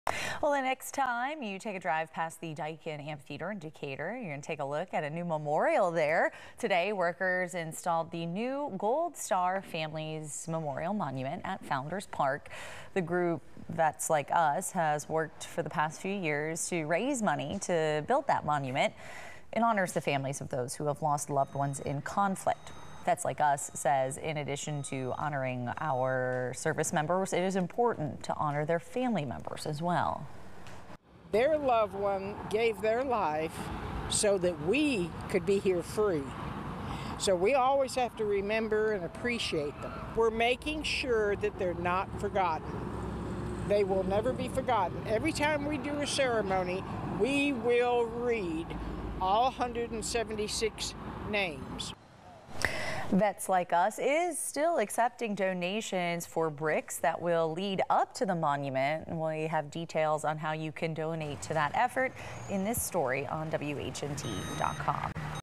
Gold Star Family Memorial Installed | June 6, 2023 | News 19 at 5 p.m.
A new memorial was installed in Decatur.